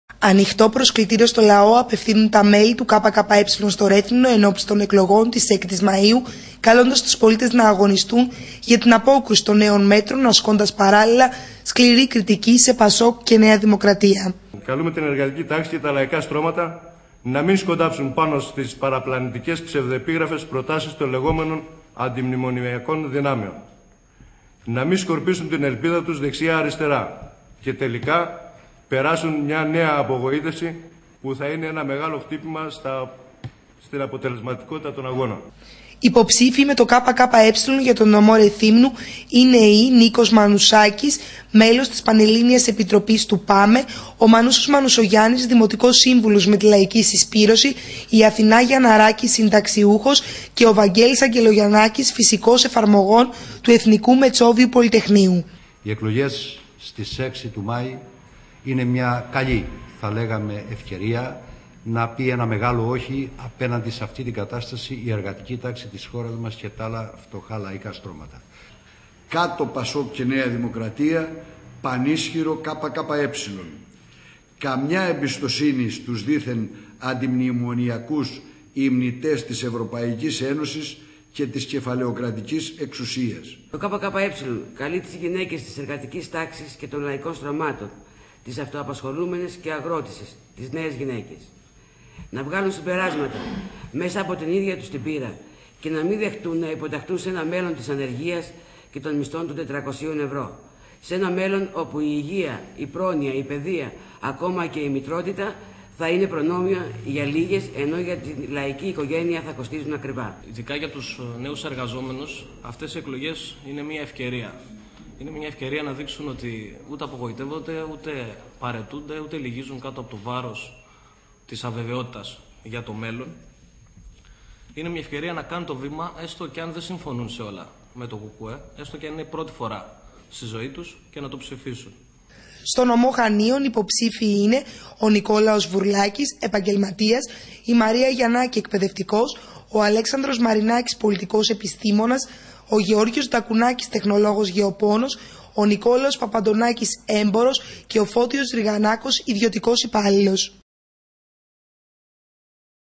ΡΕΘΥΜΝΟ ΚΚΕrn_kkeypopsifioi_115,85.flv
Παρουσιάστηκε το ψηφοδέλτιο του ΚΚΕ για τον νομό Ρεθύμνου